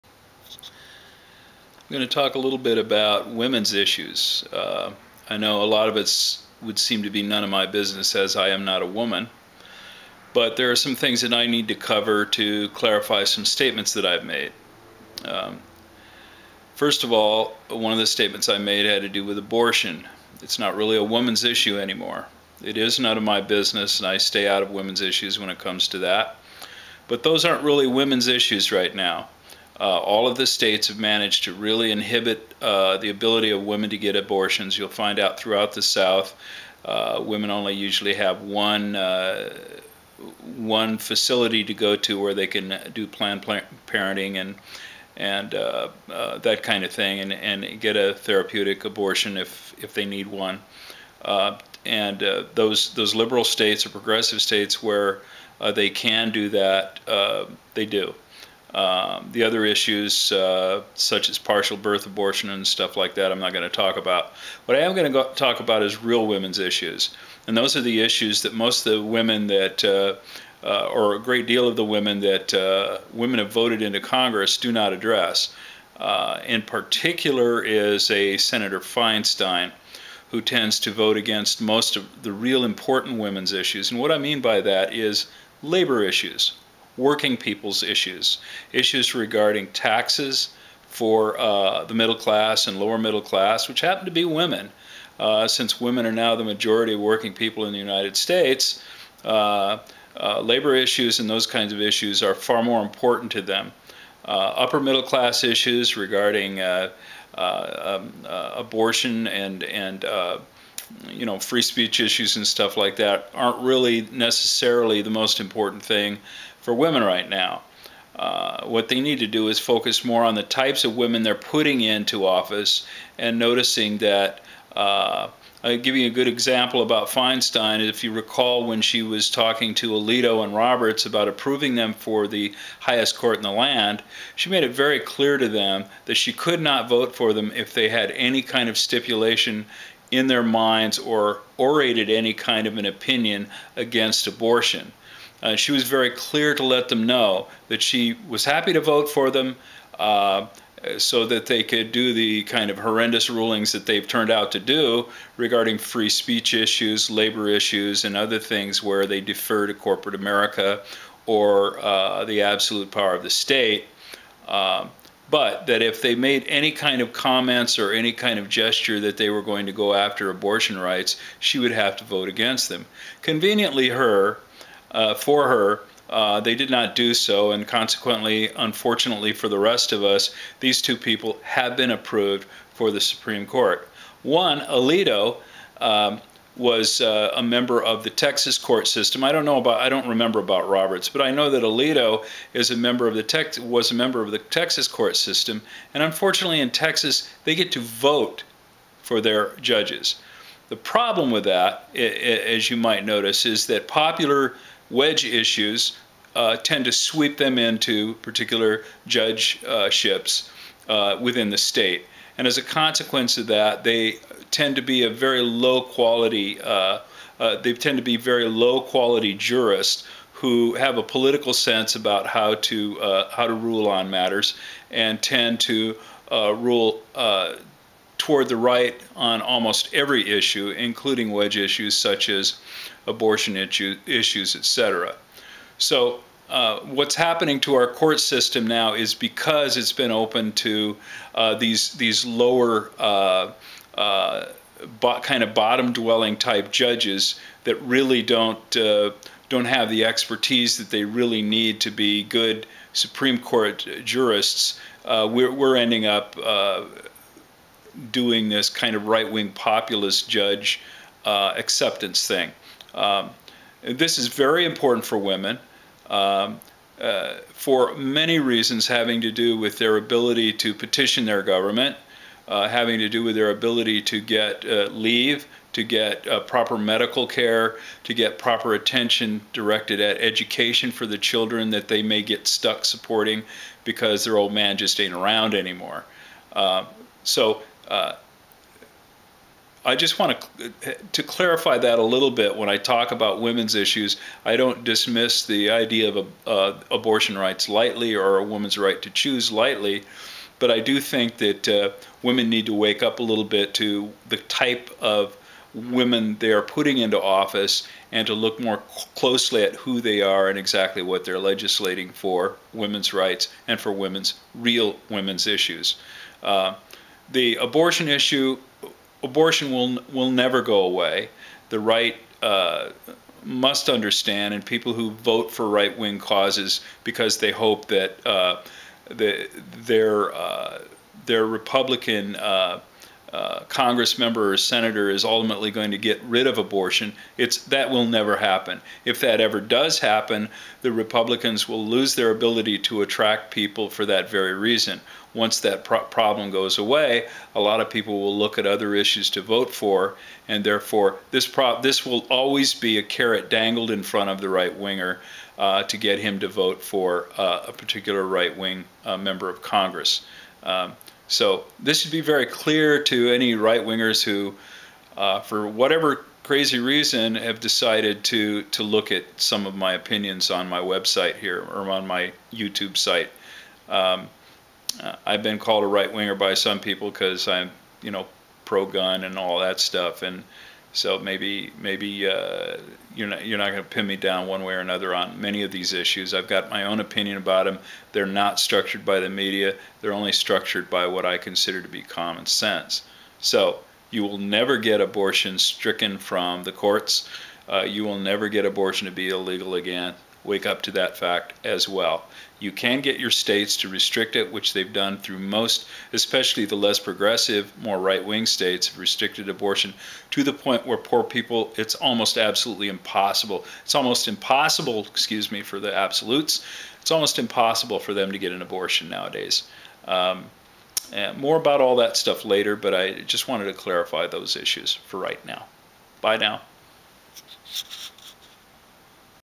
Womens Issues: Getting little or no help from so called liberal women in congress.
Real women's issues, and how few of them are being addressed by many of today's women in Congress. How some have used them to further their corporate 'liberal' agenda, and what it means to working women and their government. Poor women have had any 'choices' about their bodies put far out of their reach.